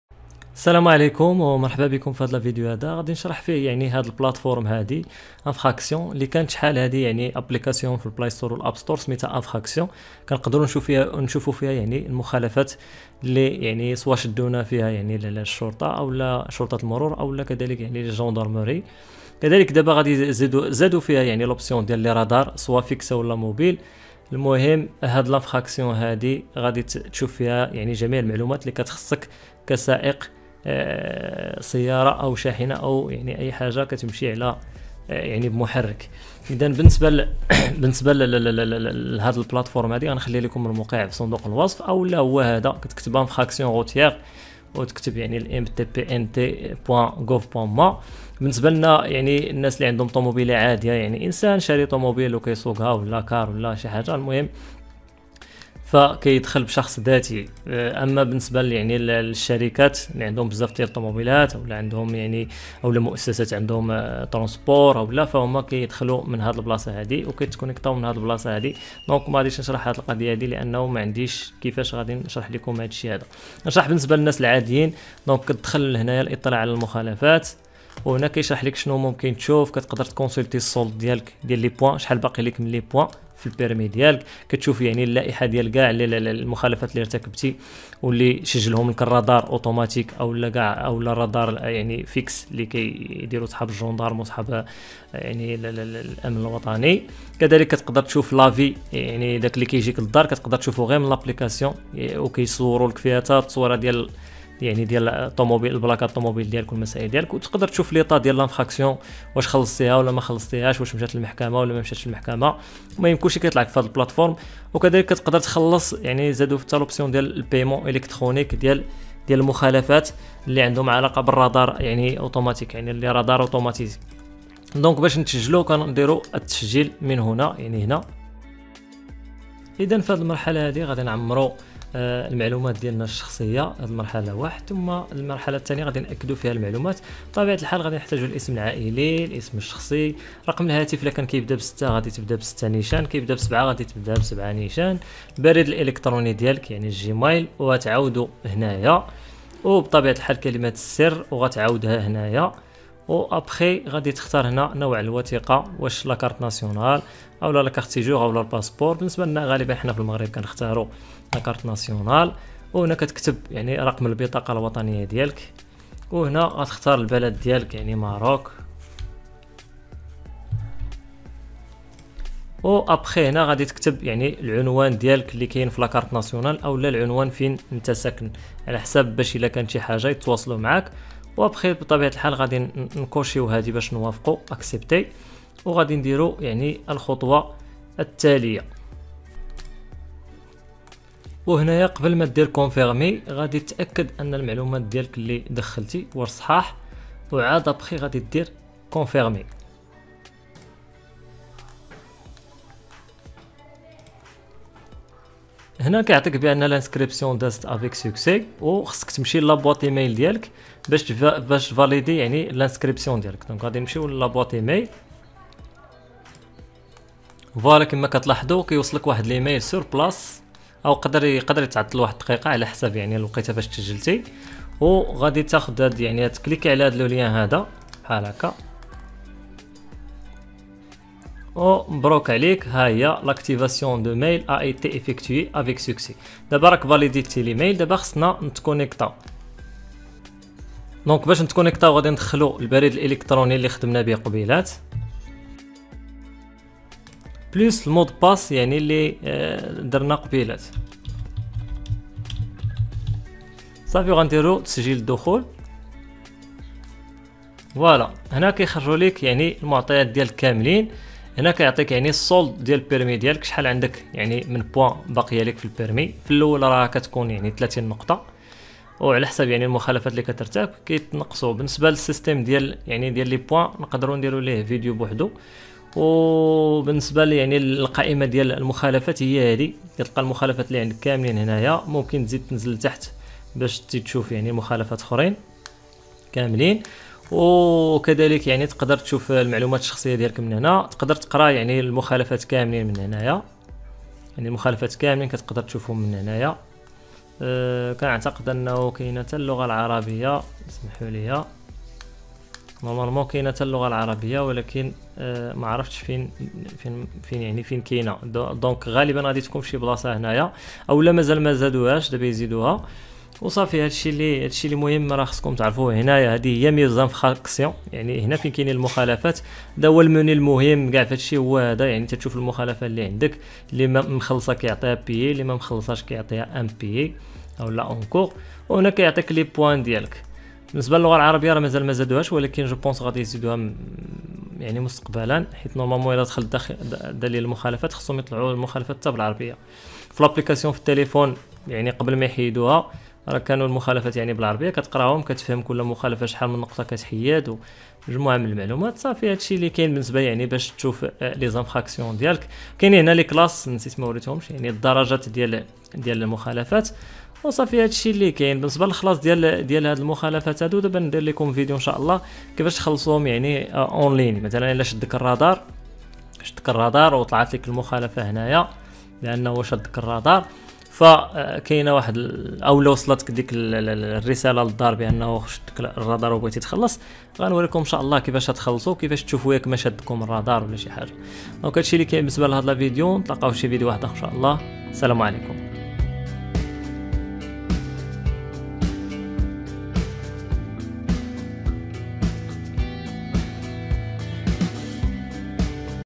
INFRACTIONS ROUTIERES طريقة الاطلاع و اداء مخالفات قانون السير
فيديو جديد فيه طريقة الاداء

INFRACTIONS ROUTIERES

La plateforme INFRACTIONS ROUTIERES vous permet de bénéficier des services sécurisés et personnalisés suivants :
La consultation de votre solde de points (pour les titulaires de permis de conduire marocains) ; Lire la suite
La liste des infractions routières enregistrées par les radars automatisés ainsi que celles constatées par la Gendarmerie Royale et la Sûreté Nationale ;
La visualisation de l’avis de la contravention ainsi que la photo enregistrée par radar automatisé ;
La consultation de l’état de l’infraction (Amende payée ou non, PV transmis au tribunal, jugement prononcé ou pas) ;
Le paiement électronique des amendes relatives aux infractions enregistrées par les radars automatisés.

تتيح هذه المنصة السائقين من الاستفادة من مجموعة من الخدمات الآمنة والشخصية وذلك على الشكل التالي:

الاطلاع على رصيد النقط الخاص برخصة السياقة (لحاملي رخص السياقة المغربية) ؛ اقرء المزيد
لائحة مخالفات قانون السير المرصودة بواسطة الرادرات الآلية أو المسجلة من قبل مصالح المراقبة التابعة للدرك الملكي وللأمن الوطني ؛
الاطلاع على إشعار المخالفة وكذلك على الصورة المرصودة بواسطة الرادار الآلي ؛
وضعية سجل المخالفات (غرامة مؤداة أم غير مؤداة ، إرسال المحضر إلى المحكمة ، صدور الحكم) ؛
الأداء الإلكتروني للغرامات المتعلقة بالمخالفات التي تم رصدها بواسطة الرادارات الآلية.